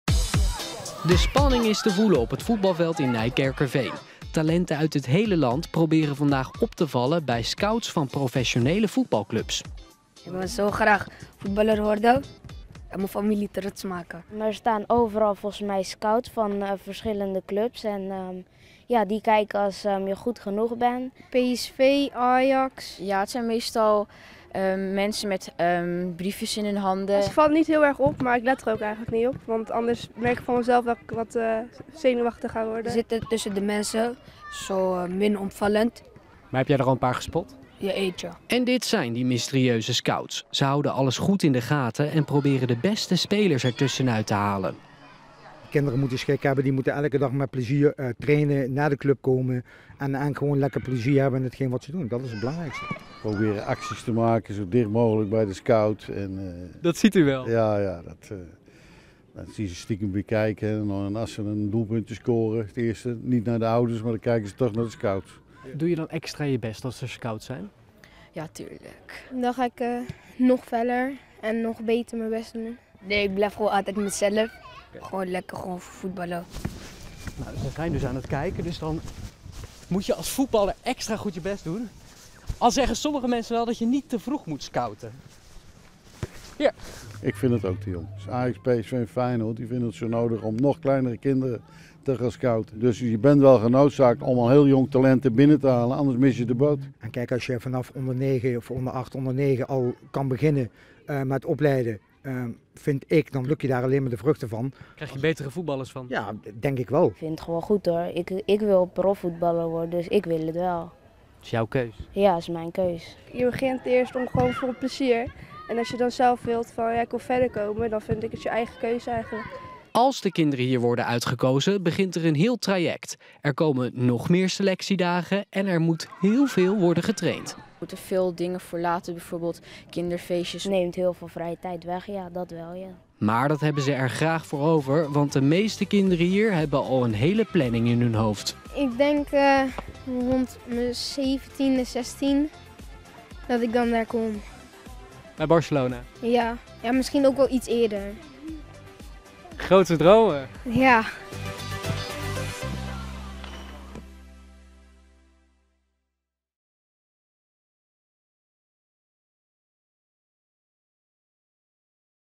Deze kinderen willen ontdekt worden als profvoetballer
Voor veel jongens en meisjes is het een grote droom: profvoetballer worden. Maar bij sommige amateurclubs komt er nooit een talentenscout langs. Daarom is er iets nieuws bedacht: een talentendag waarvoor iedereen zich kan opgeven.